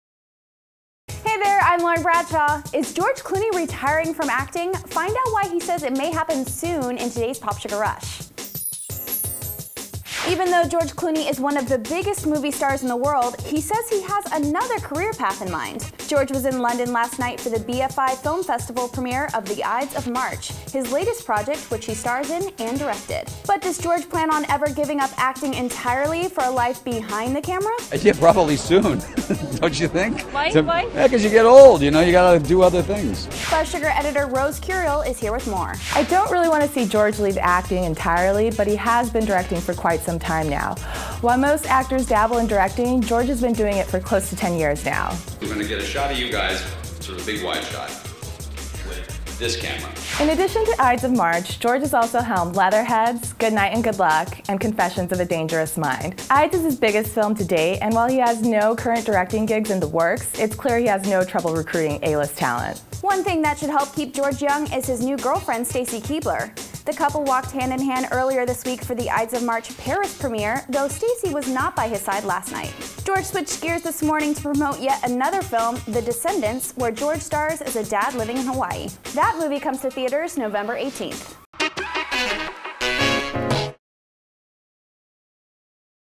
George Clooney May Quit Acting Soon
George Clooney worked the red carpet at the London premiere for The Ides of March last night. George directs and stars in the film, and he says he may quit acting soon, so he can spend more time behind the camera! Check out the interview in today's PopSugar Rush.